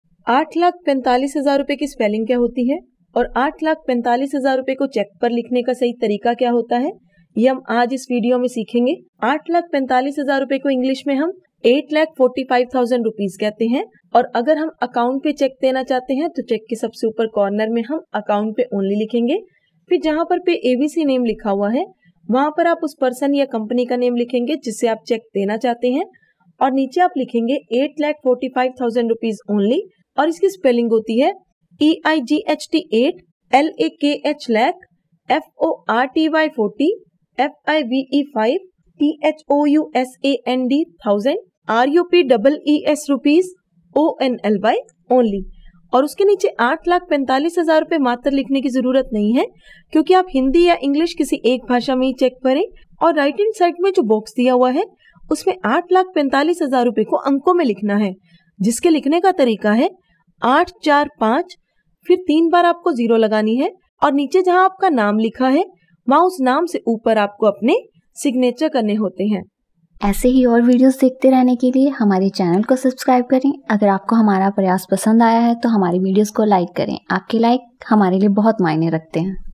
845000 ko english mein kya kahate hain | 845000 in words | 845000 ki English | 845000 spelling
845000 ko english mein kya kahate hain | 845000 in words | 845000 ki English | 845000 spelling

तो बस एक मिनट में सीखिए 845000 को cheque par kaise likhe
Let’s learn how to write 845000 in English.

845000 का अंग्रेजी में अर्थ होता है
845000 रुपये को Cheque पर लिखने का सही तरीका :

Eight lakh forty five thousand Rupees only
आठ लाख पैंतालीस हज़ार रुपये मात्र

845000/-

account payee